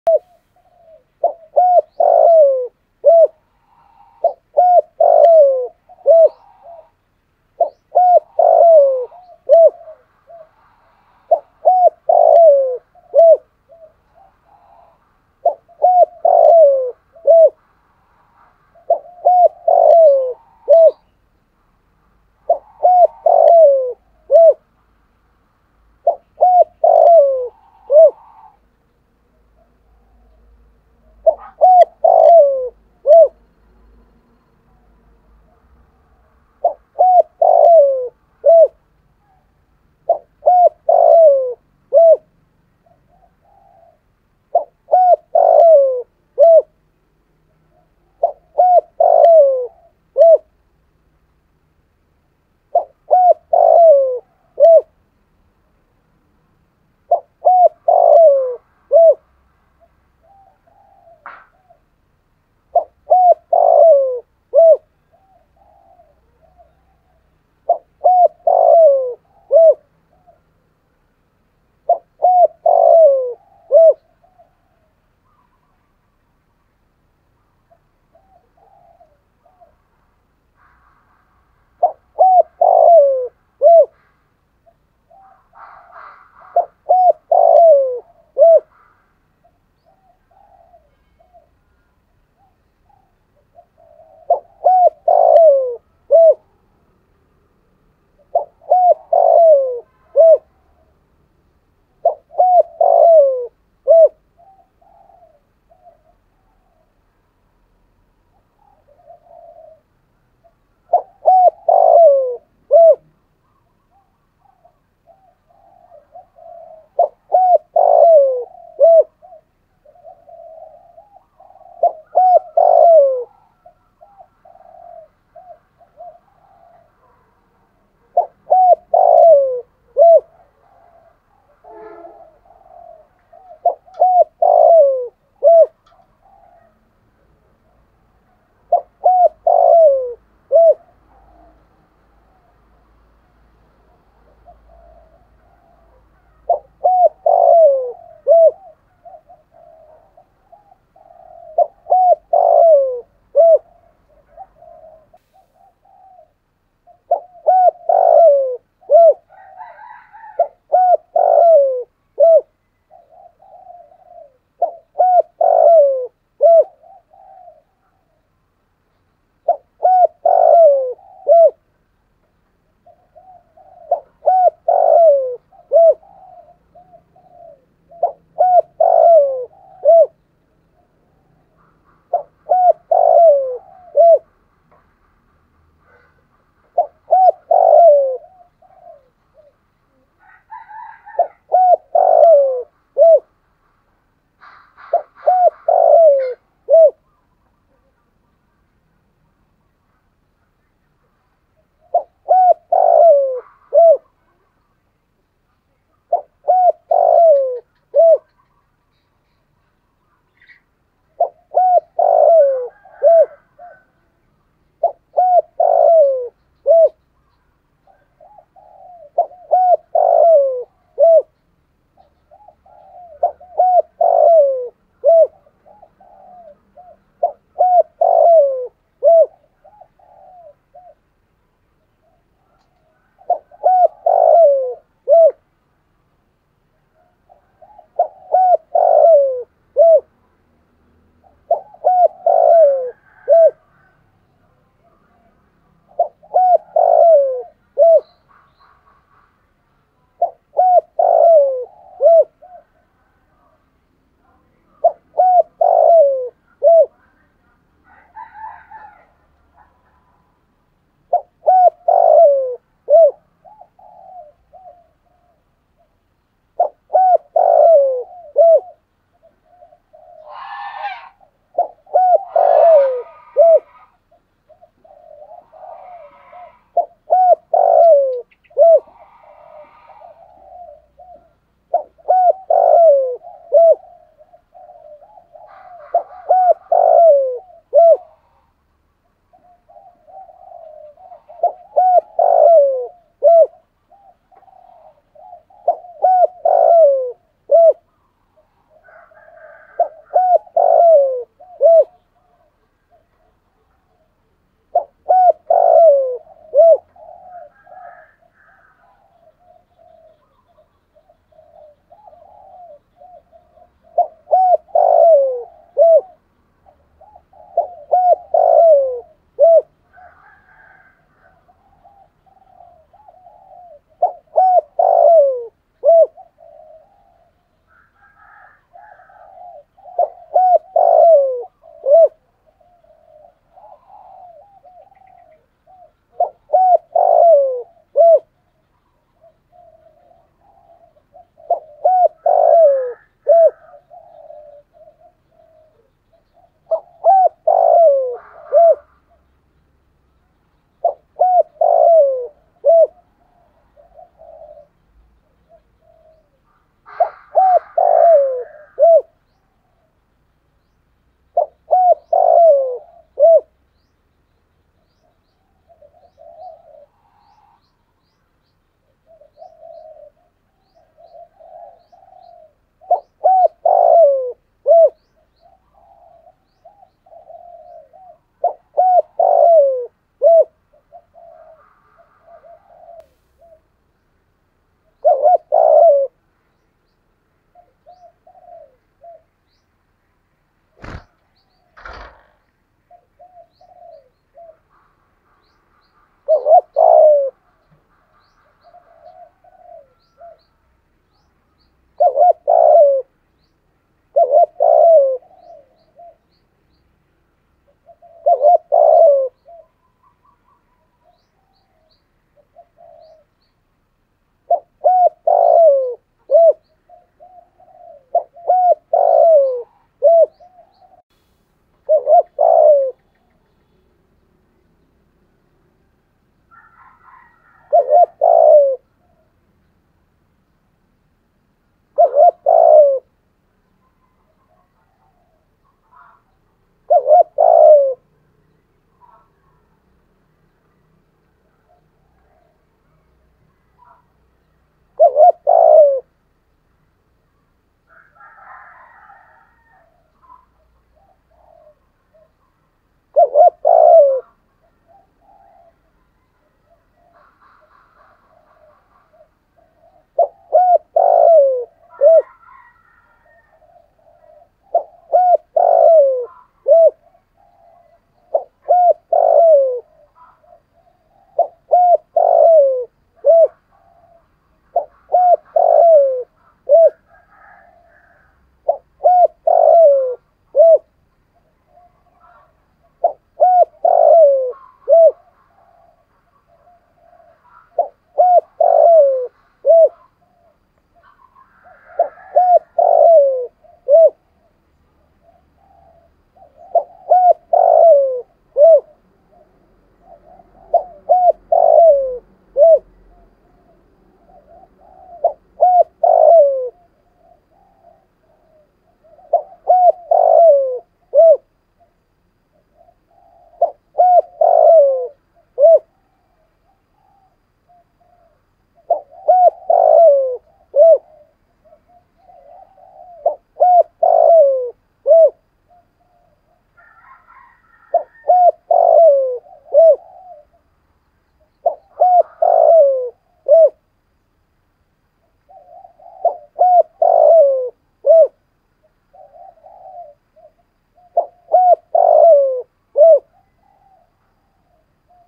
cu gáy giọng thổ, tiếng gáy gọi giọng to rõ, ít tạp âm
cu gay giong tho cuc dinh gáy gọi bổi lúc sáng sơm, vdeo được chúng tôi ghi hình và biên tập + xuất bản trên kênh mới mỗi ngày để anh em dùng kích bổi tập mồi hiệu quả